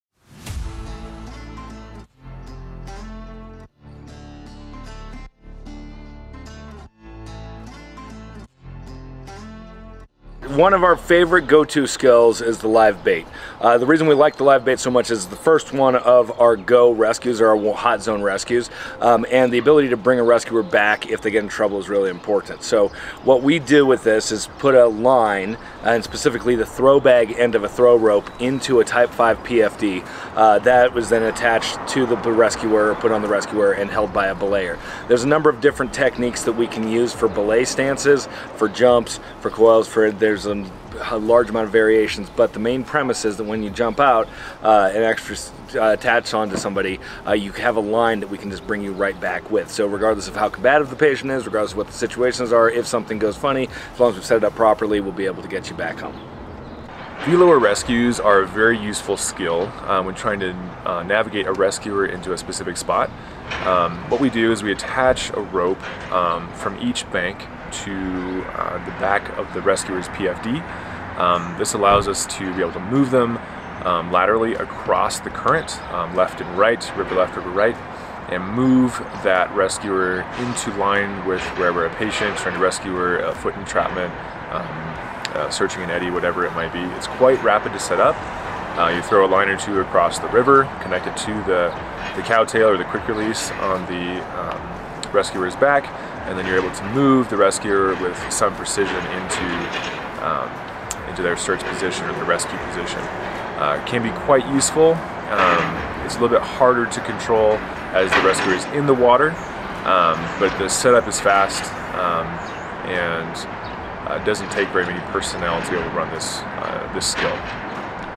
TRUE RESCUES - Swiftwater Rescue Skills
This video is a part of our Swiftwater Rescue Skills series, offering explanations and tutorials for every Rescue3 SRT and SRT-1 skill. These skills are performed as a part of the International Rescue and Relief student training with instruction by Rescue3 instructors.